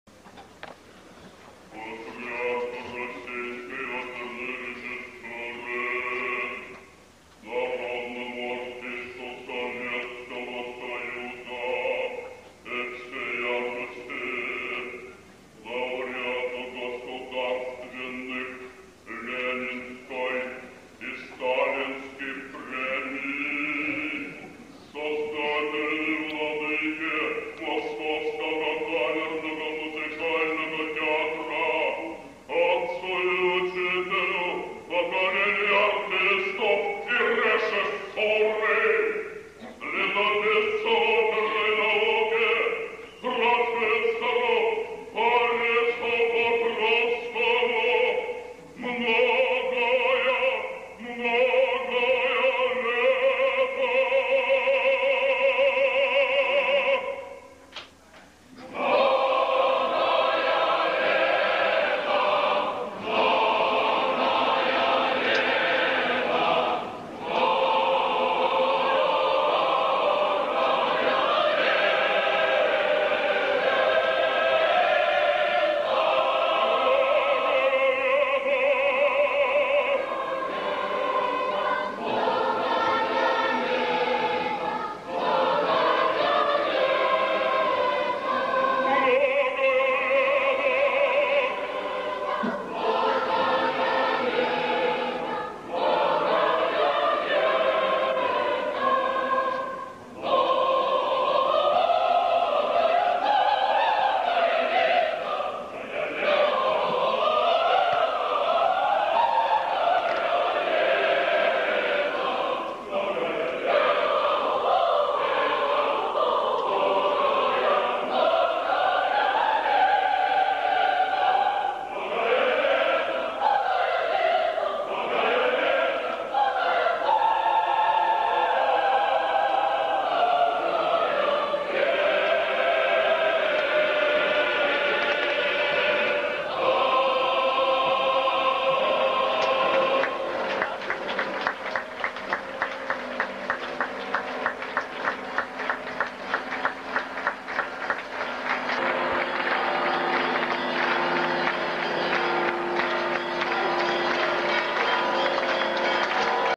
Vladimir Matorin - Pokrovsky's - 85 4/5
Boris Pokrovsky's - 85
Vladimir Matorin
Chorus e Orchestra  Bolshoi theater
Conductor - Stanislav Lykov
Bolshoi Theater 23/ 01/ 1997